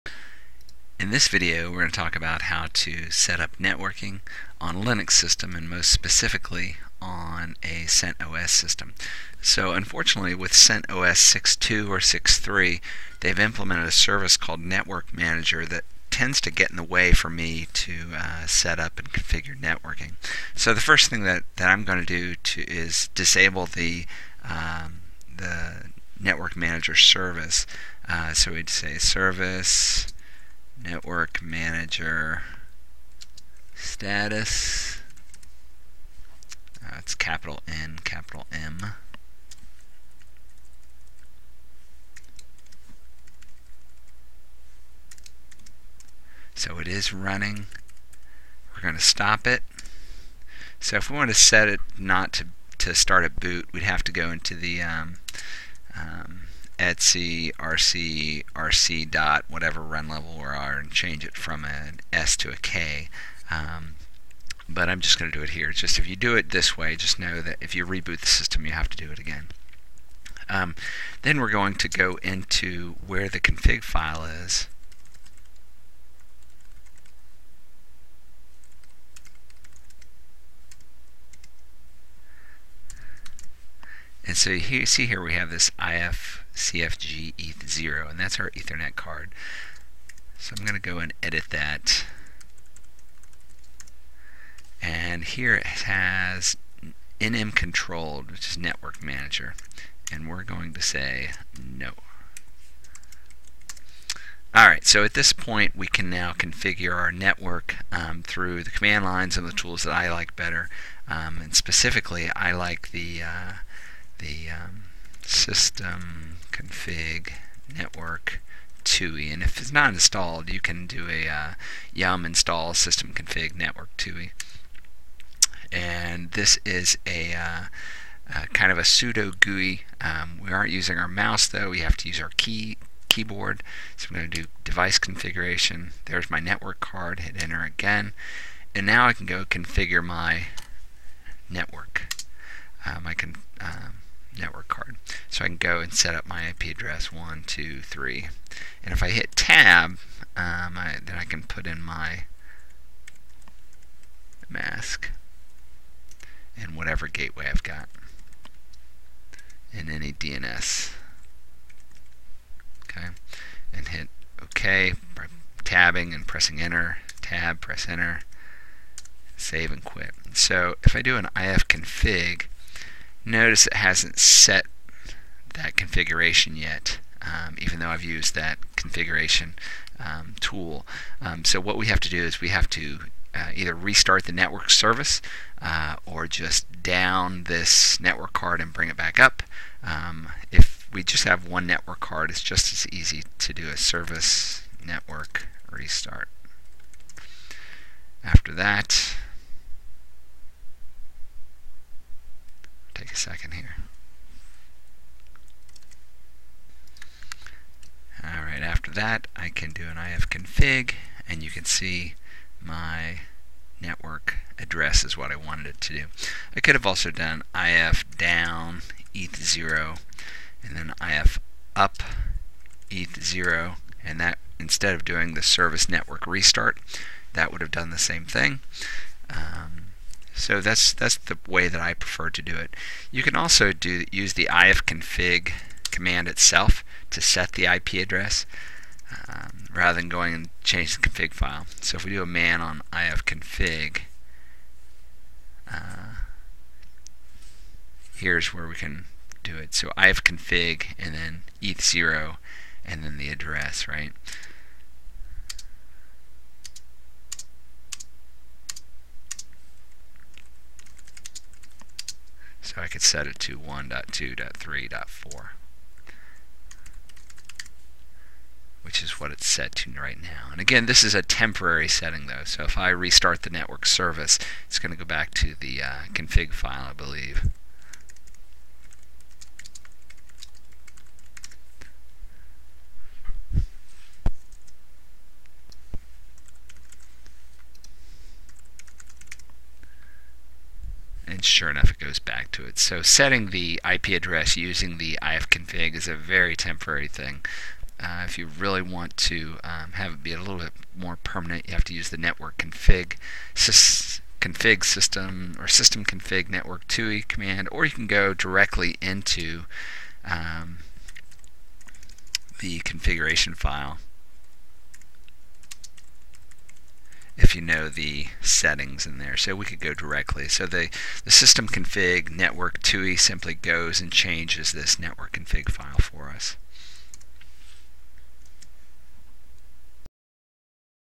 Linux (CentOS) Network Setting
Remove that pesky NetworkManager and use system-config-network-tui again.  Woot!